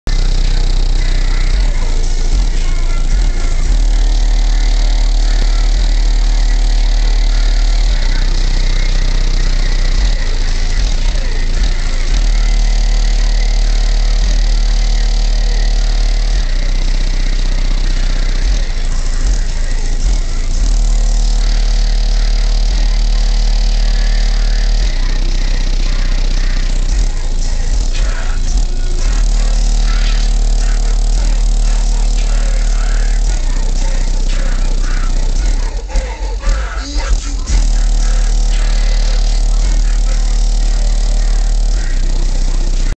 ben sales door flex
the car has 8 6 & 1/2 on each door 6 12 incriminator audio death Penalty and 8 6 & 1/2 on the wall a 6 & 9 on the dash